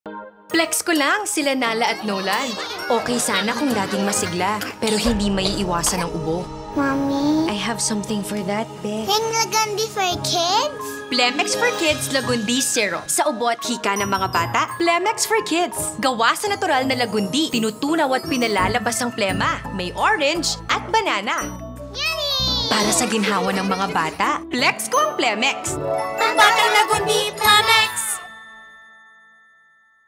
Plemex for Kids "PLEX"
Plemex For Kids TV material featuring Camille Prats-Yambao and kids Nala and Nolan for Trevenodd Corp.

Client: Trevenodd
Project: "Plex"
Production House: Bread and Butter Creative Productions
Director: Mike Plana
Producer/Creative Director: Joey Feir
DOP: Monchie Redoble
AD: Roni Bertubin
PD: Aida Concepcion
Propsman: Darius Espinosa
Production Manager: Sheila Cusi
Offline/Online Editor: Jojo Medalla
Color Grading: Peter Williams for Smoke and Mirrors, Bangkok